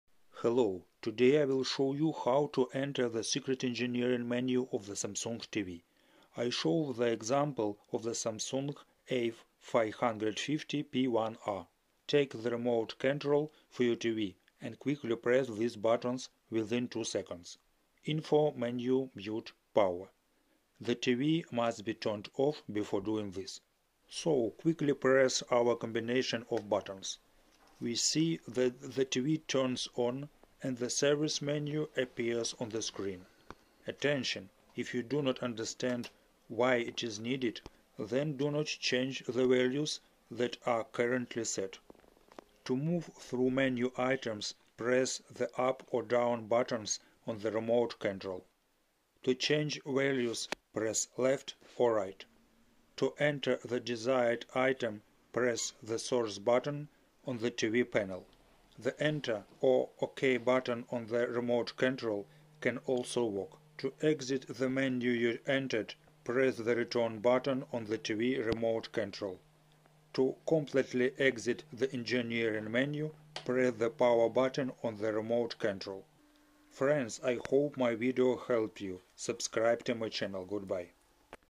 How to enter the engineering menu of SAMSUNG TV Instructions ✔️
What is a service (engineering) menu of SAMSUNG TV ? This menu is for setting the operating system boot method. This menu determines options such as display size and type; determines what voltage should be supplied to the screen backlight, which modules are active and which are not, controls the output power of the audio amplifier, and much more.
If you set the wrong display type, the image may be upside down.